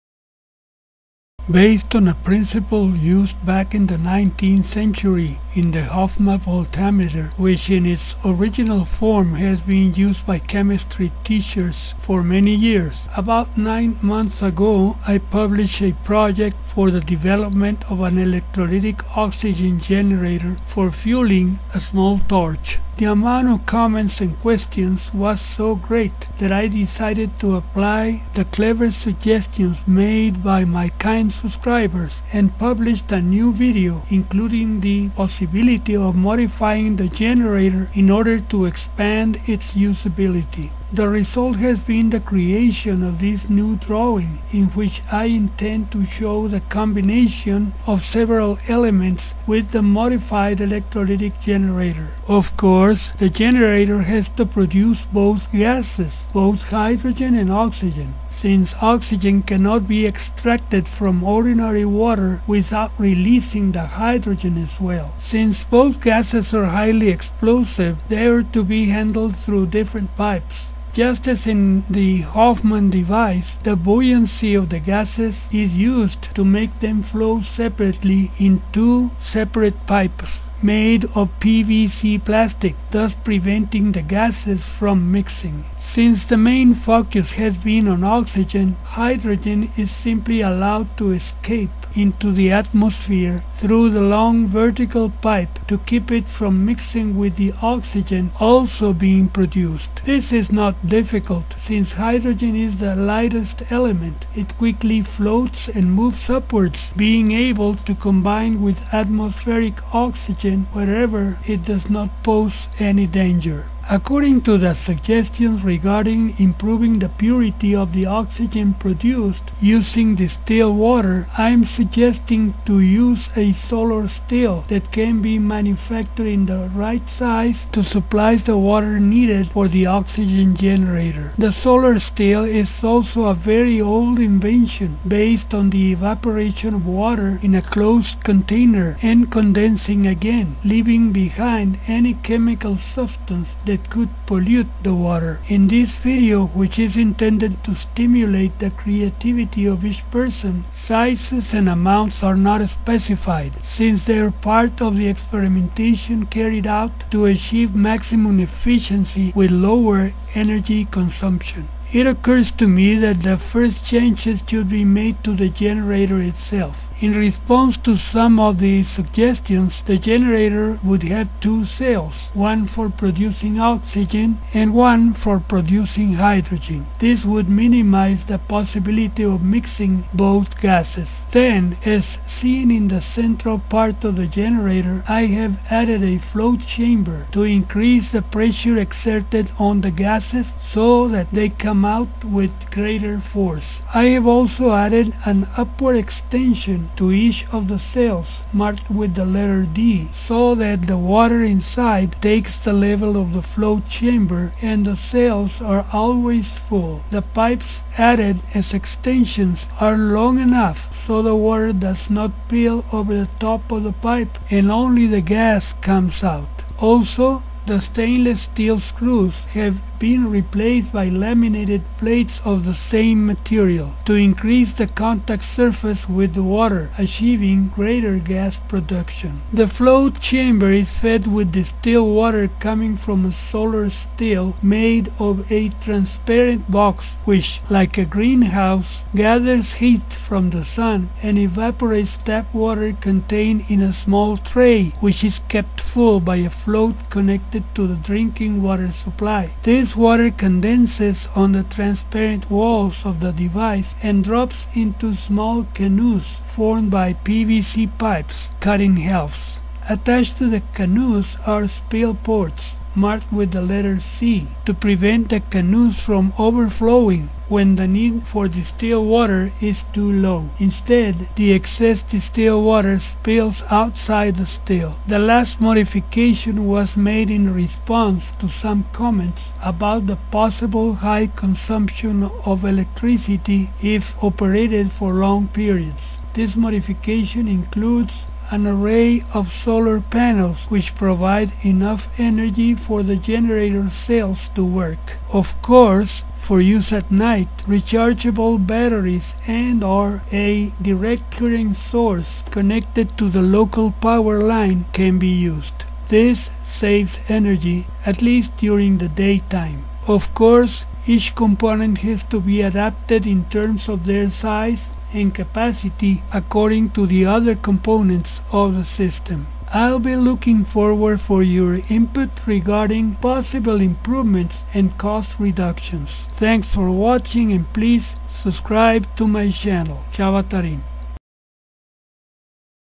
Suggestions for improving oxygen generator
Due to the numerous positive comments about my project on an oxygen generator, I am posting this video with some improvements suggested by my subscribers. . Improvements and reason for them are in My Community , where I show an update of the drawing as a result of their suggestions. Please help by contributing some ideas!